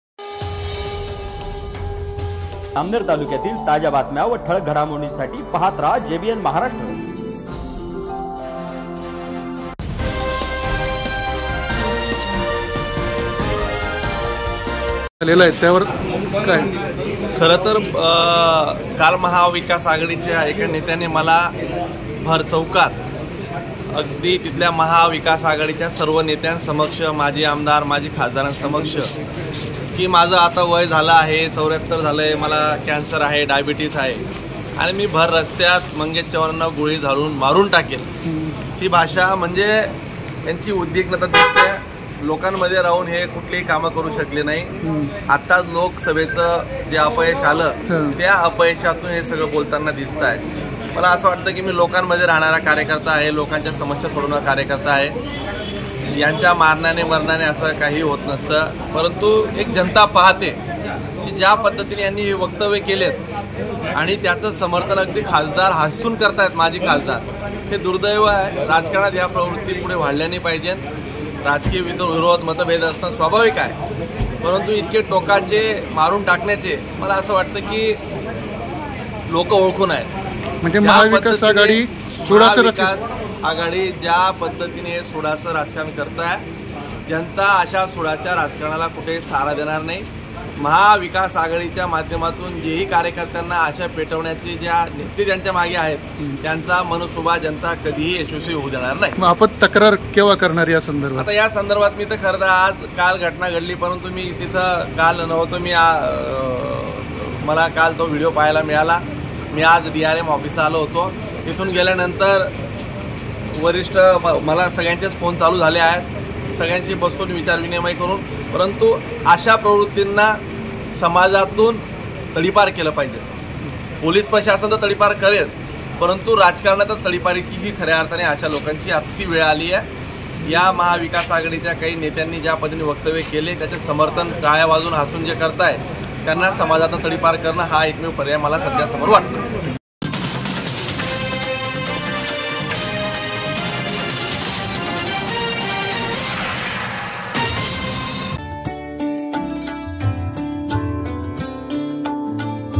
चाळीसगावचे आमदार मंगेश चव्हाण यांना दिली जीवे ठार मारण्याची धमकी
JBN Maharashtra News Jalgaon 1